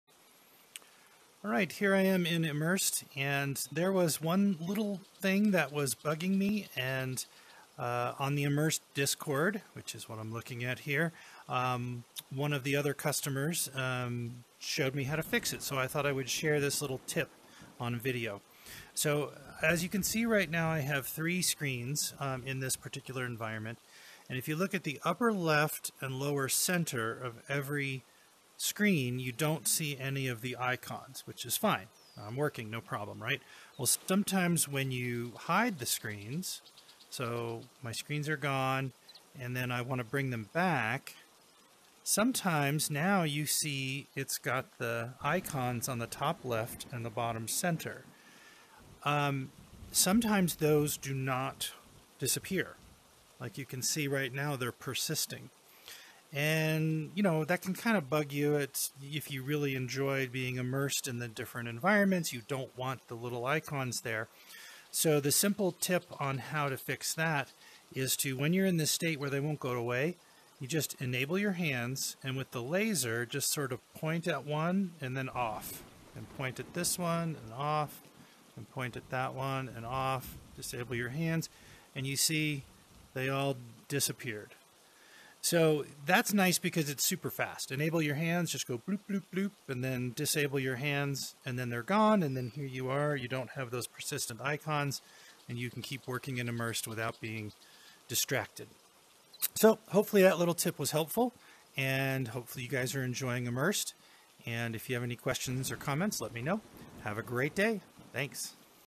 Immersed - Tips & Tricks - Persistent Icons
These "Tips & Tricks" videos demonstrate little things I've learned that either fix a problem or help me work more efficiently in Immersed.  In this video, I demonstrate what to do if the screen icons won't go away.